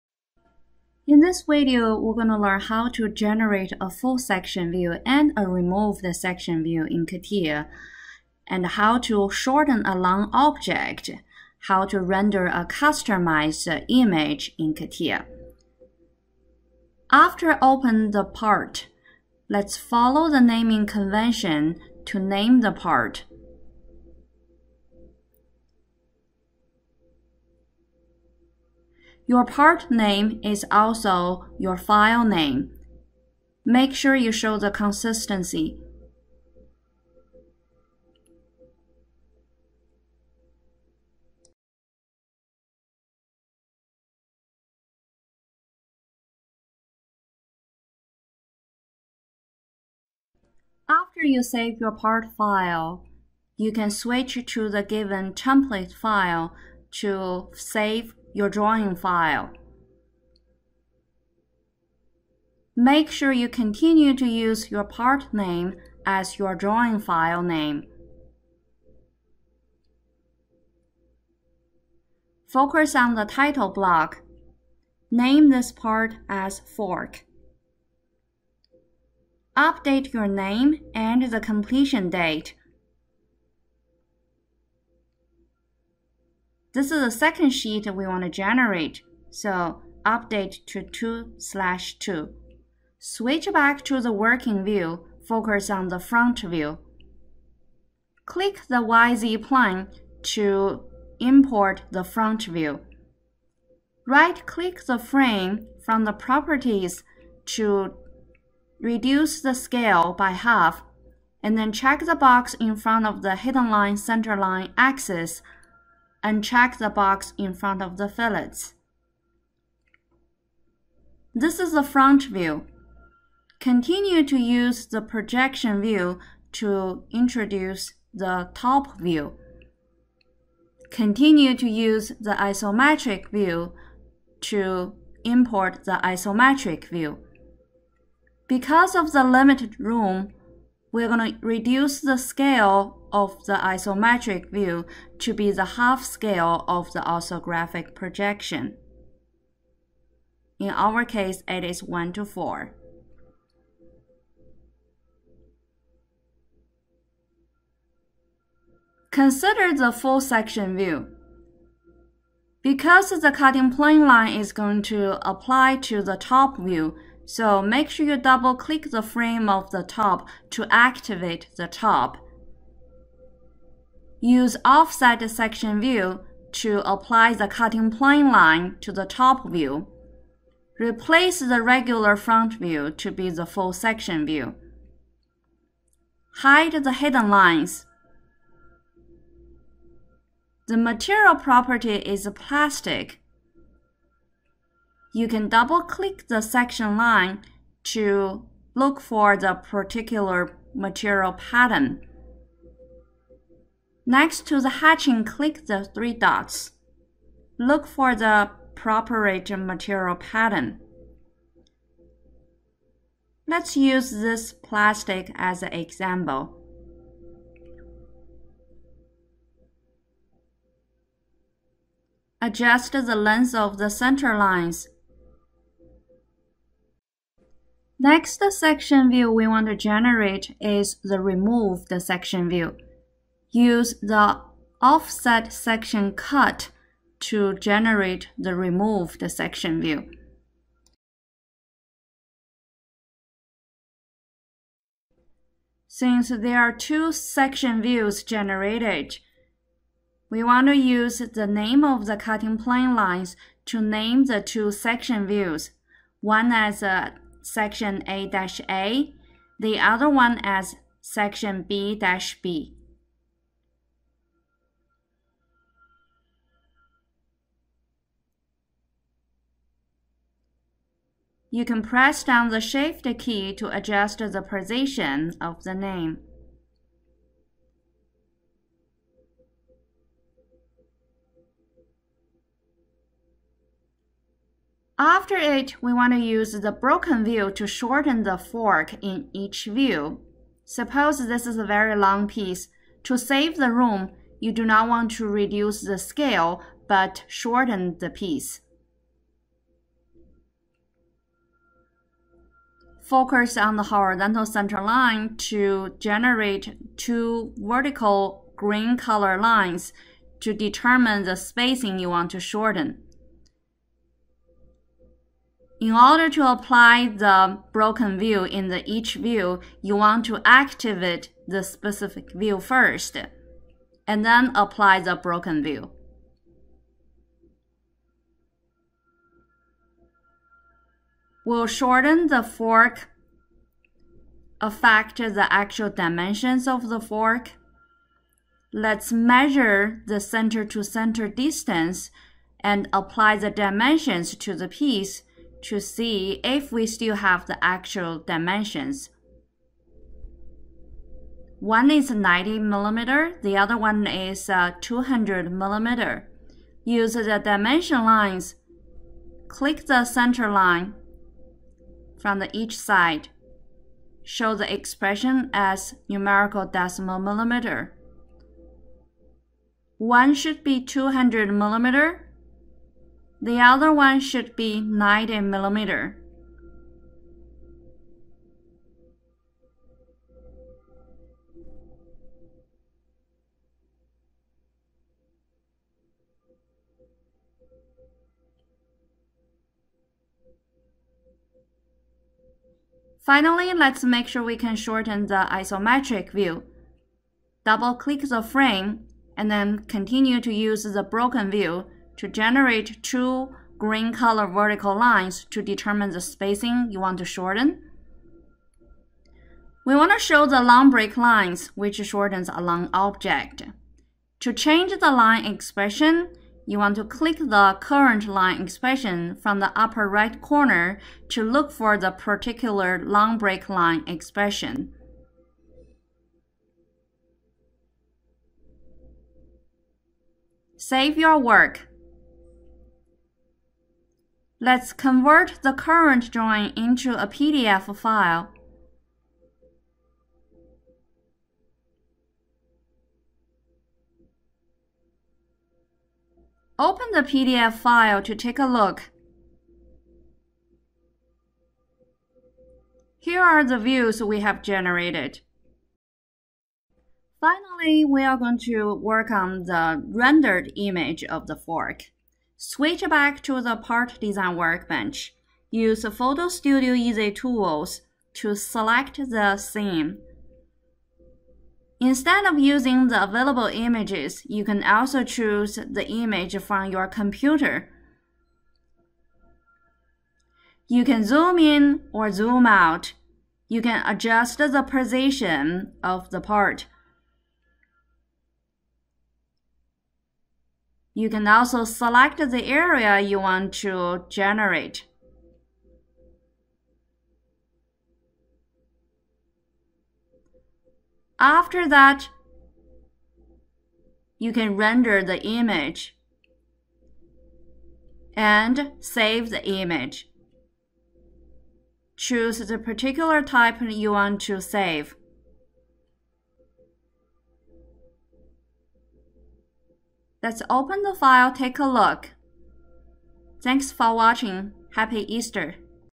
Full section and removed section view in CATIA
This video shows how to create a full section and a removed section views in CATIA, It also shows how to shorten a long piece and how to render a part with a customized background